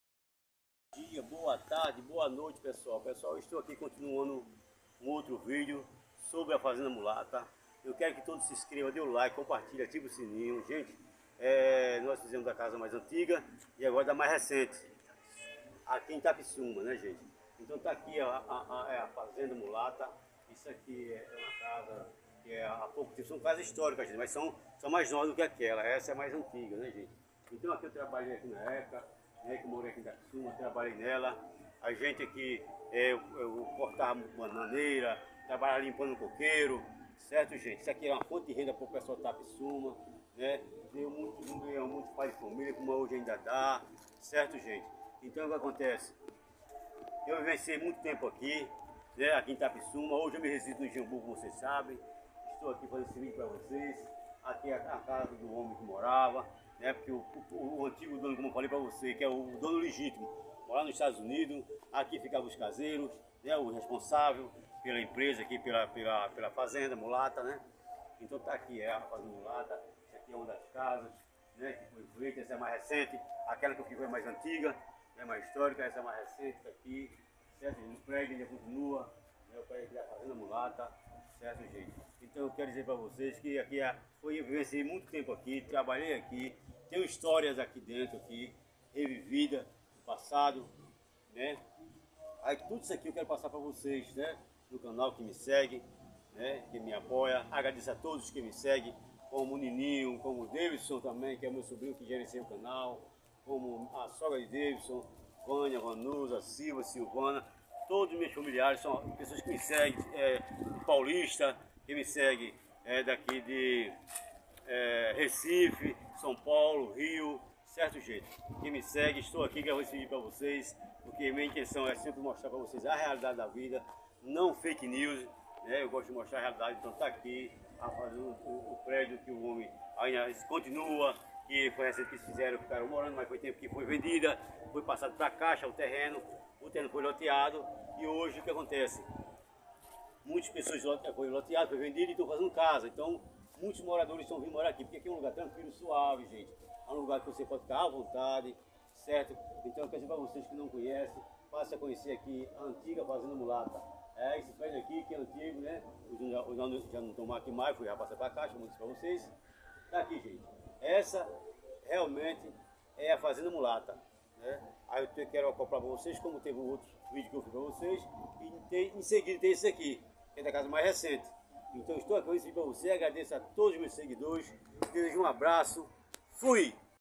CASARÃO DA ANTIGA FAZENDA MULATA | ANTIGA CONSTRUÇÃO | ENGENHO DE AÇÚCAR | ITAPISSUMA PERNAMBUCO
Faça um comentário, dê sugestões de vídeos 😊